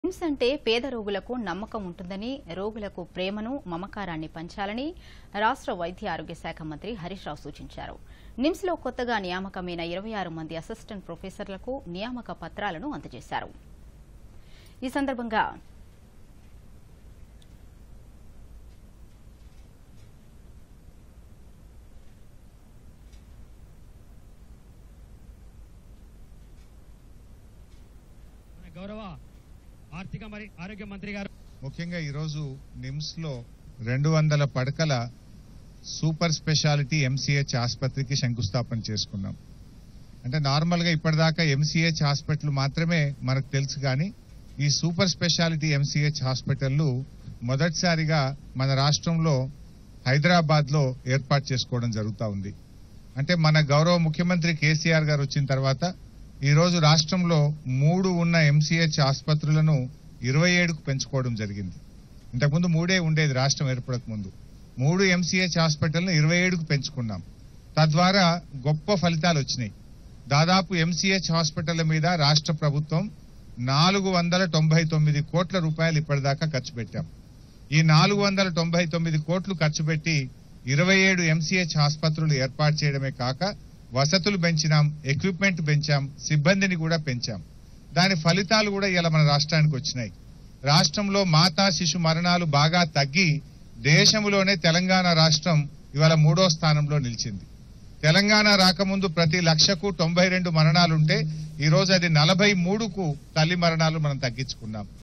నిమ్స్ అంటే పేద రోగులు నమ్మవచ్చు | Health Minister Harish Rao | 🟥 DD News Telangana
నిమ్స్ అంటే పేద రోగుల‌కు న‌మ్మ‌కం ఉంటుంద‌ని,  రోగులకు ప్రేమ‌ను, మమ‌కారాన్ని పంచాల‌ని రాష్ట్ర వైద్యారోగ్య శాఖ మంత్రి హ‌రీశ్‌రావు సూచించారు. నిమ్స్ లో కొత్త‌గా నియామ‌క‌మైన 26 మంది అసిస్టెంట్ ప్రొఫెస‌ర్ల‌కు నియామ‌క పత్రాల‌ను అంద‌జేశారు.
ఈ సంద‌ర్భంగా మంత్రి హ‌రీశ్‌రావు మాట్లాడుతూ.. నిమ్స్ వంటి ప్ర‌తిష్టాత్మ‌క‌మైన సంస్థ‌లో ఉద్యోగంలో చేర‌డంలో ఎంతో గౌర‌వాన్ని ఇస్తుందని చెప్పారు. నిమ్స్ లో ప‌ని చేయ‌డం అంటే.. ఇక్క‌డ చేయ‌గ‌లిగిన‌న్నీ కేసులో ఏ కార్పొరేట్ హాస్పిట‌ల్‌లో కూడా చేయ‌లేరన్నారు. నిమ్స్ లో ఫస్ట్ టైమ్ అసిస్టెంట్ ప్రొఫెస‌ర్ నియామ‌కాల్లో ఎస్సీ, ఎస్టీ, ఓబీసీ, వుమెన్, ఈడ‌బ్ల్యూఎస్ రిజ‌ర్వేష‌న్లు అమ‌లు చేశామని తెలిపారు. ఇంత వ‌ర‌కు నిమ్స్ లో ఎలాంటి రిజ‌ర్వేష‌న్లు ఉండేది కాదన్న ఆయన... సీఎం కేసీఆర్ సూచ‌న మేర‌కు అన్ని వ‌ర్గాల‌కు అవ‌కాశం క‌ల్పించాల‌నే ఉద్దేశంతో ఈ విధానాన్ని అమ‌లు చేశామని చెప్పారు. రాష్ట్రంలో సూప‌ర్ స్పెషాలిటీ అందించేం వైద్యం నిమ్స్ లో మాత్ర‌మే ఉందని..  అందుకే రాష్ట్రంలోని ప్ర‌తి మారుమూల ప‌ల్లె నుంచి రోగులు ఇక్క‌డికి వ‌స్తుంటారు కాబ‌ట్టి రోగుల‌కు నిమ్స్ ప‌ట్ల‌ ఉండే న‌మ్మ‌కాన్ని కాపాడాలని హరీశ్ రావు సూచించారు